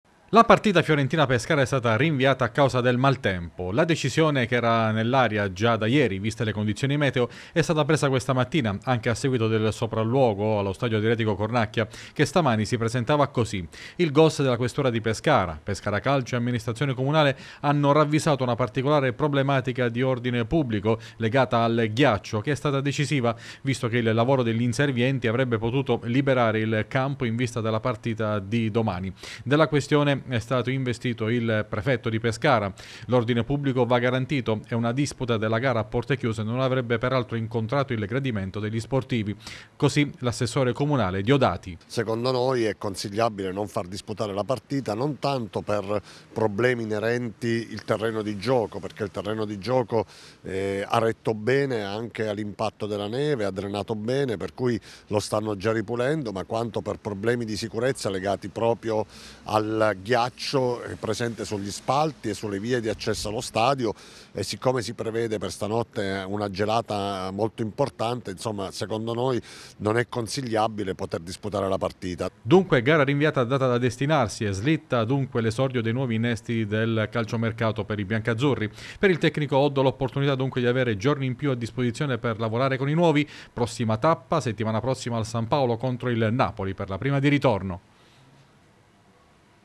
Calcio. Rinviata Pescara - Fiorentina
E’ stata rinviata la partita di Serie A tra Pescara e Fiorentina. Decisione presa stamani per motivi di ordine pubblico legato alla presenza di ghiaccio sugli spalti e nelle strade adiacenti lo stadio. Ascoltiamo l’assessore comunale allo Sport Giuliano Diodati